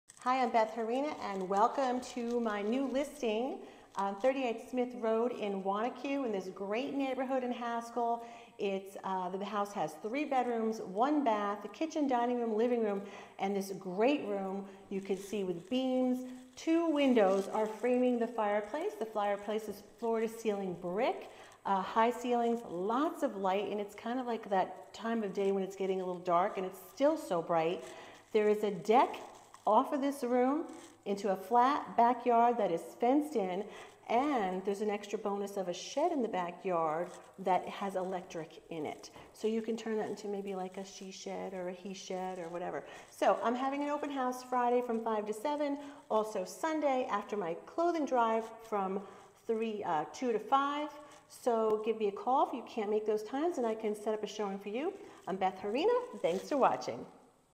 GREAT STARTER HOME WITH AMAZING GREAT ROOM ADDITION!!
Welcome home! This Cape Cod charmer offers 3 bedrooms, 1 bath, and a thoughtfully designed large addition with cathedral ceilings, beams, and a cozy fireplace. This is the perfect space to enjoy movie night or relax by the fire. Step onto beautiful hardwood floors and through sliding glass doors to a spacious deck overlooking a large, flat backyard - a canvas for creating years of cherished memories. The eat-in kitchen with ample storage and plenty of counter space opens to the side portico, a perfect way to bring in the groceries protected from the weather. The dining room or second living room features hardwood floors and a large bay window creating a warm and bright space. Upstairs discover two spacious inviting bedrooms with extra storage. The property is located in a vibrant neighborhood perfect for biking, walking and meeting your new neighbors. This property is a mere half-mile walk to Back Beach Park’s trails, playground, soccer fields & picnic area. A walk away is Wanaque Dog Park and Haskell School. Close proximity to public transportation, restaurants, pharmacy, grocery stores, 45 minutes from NYC.